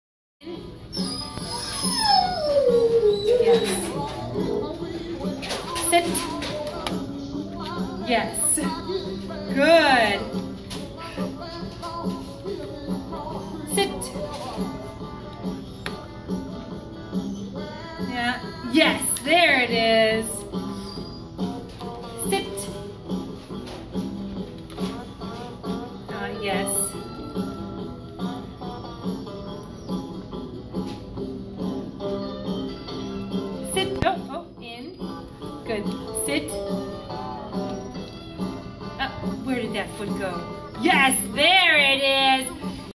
Vision: Rear End Strengthening With A Chair (H360)
Vision can do this on a book but a chair proved to high of a leap so we went to a stool.